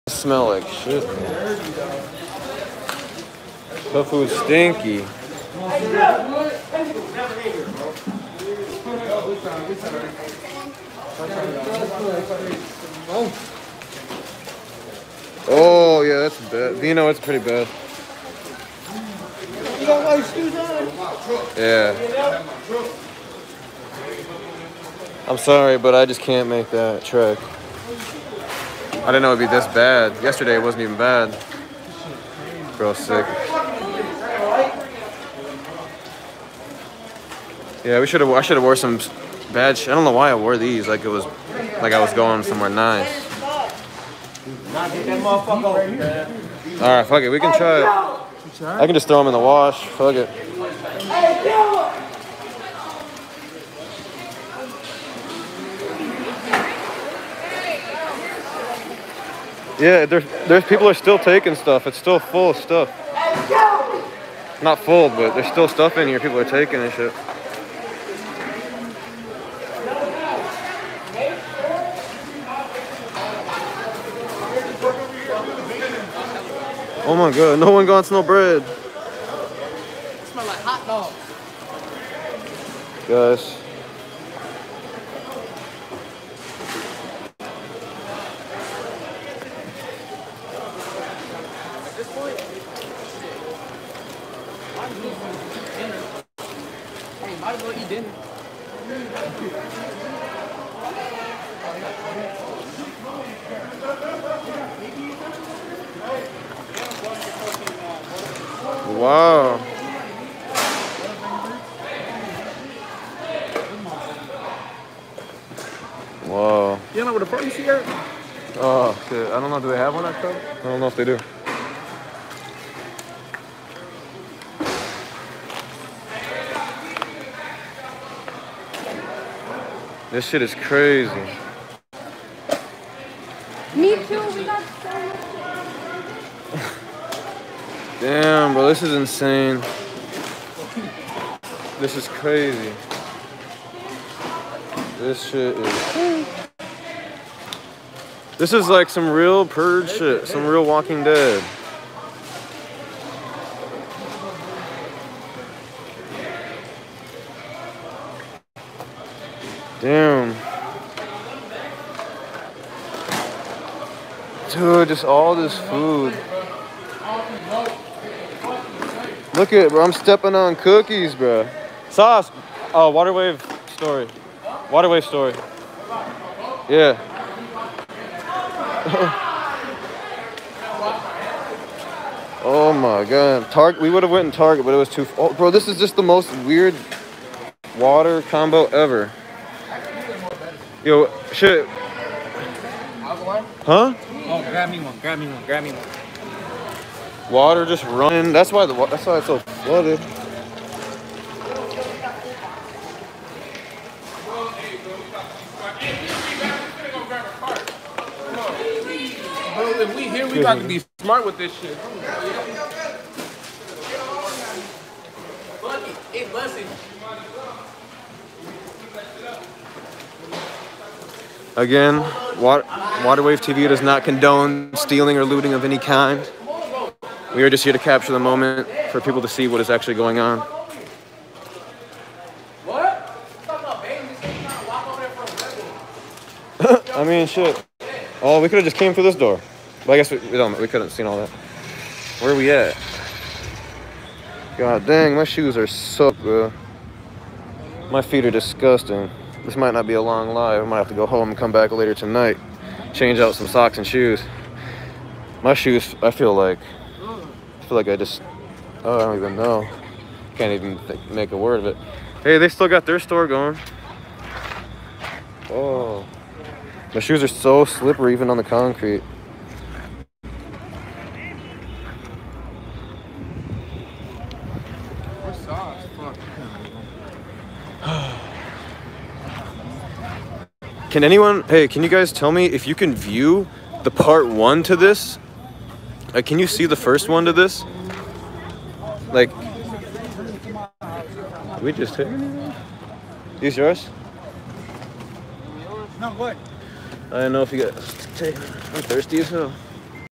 Post apocalyptic scenes from grocery store in Minneapolis
Power is out, brown sludge on the ground, smells bad according to streamer.  Cub grocery store near 3rd precinct, recorded after Thursday looting and destruction by water wave.